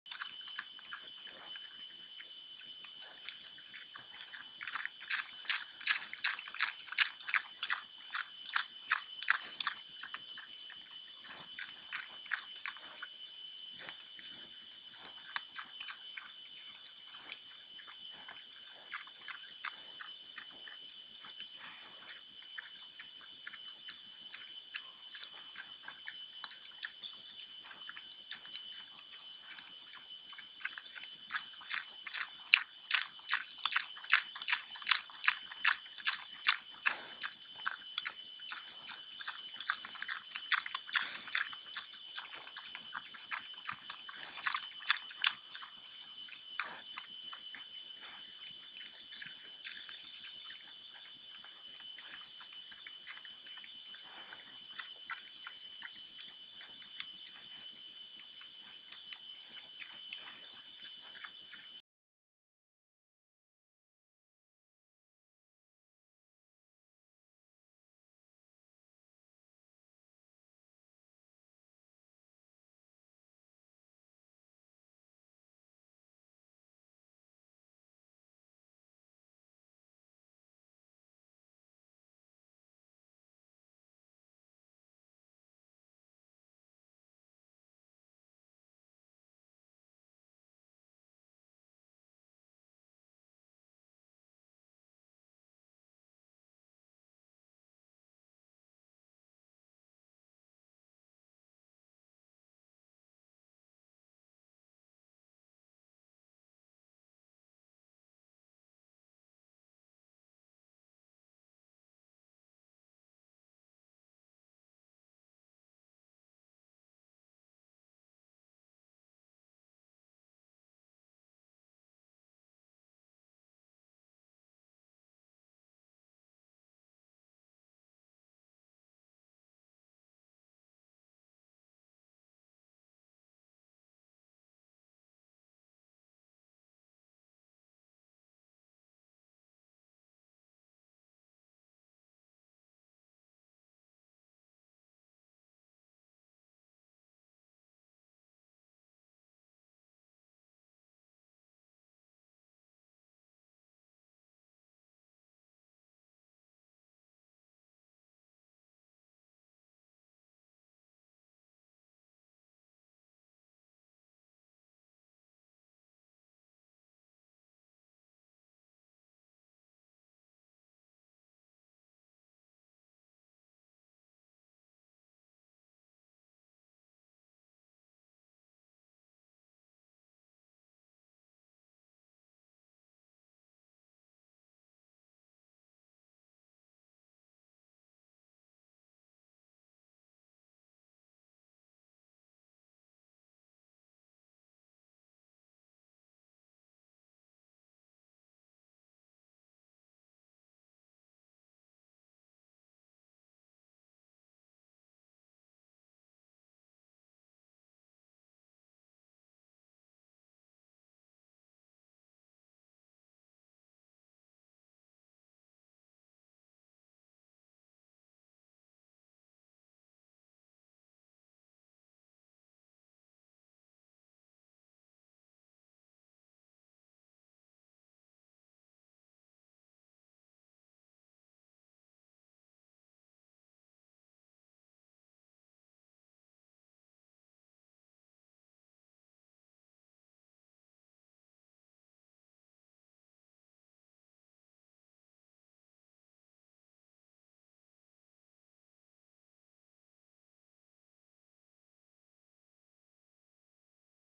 Karu-Smack-Smack-Smack-2 July 2013
I don't know what it is, but for some reason I get a real kick out of hearing those wild boars smacking as they eat!